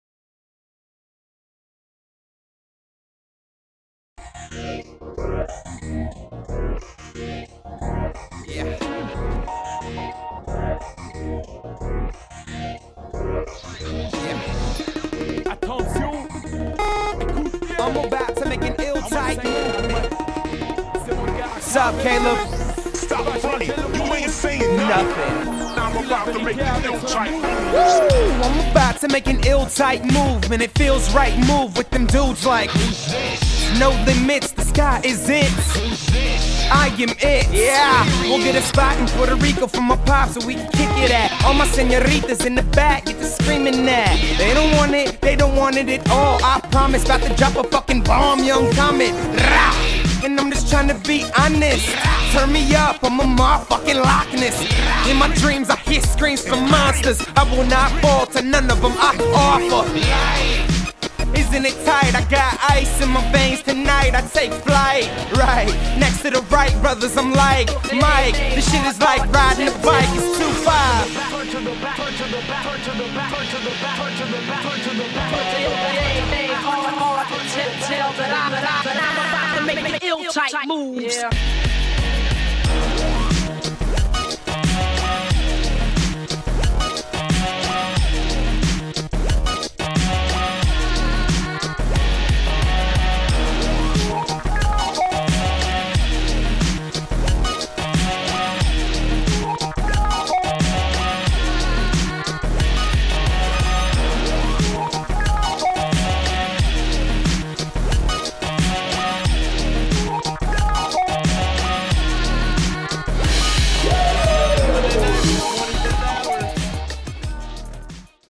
Recorded in a Frat House.
Promo video for The 25th HR., a band of college students playing out of Arizona State University

Kyle Benjamin Wolcott - Vocals & Lyrics
Brett Ettinger - Guitar & Effects
Barry Collens - Drums & Percussion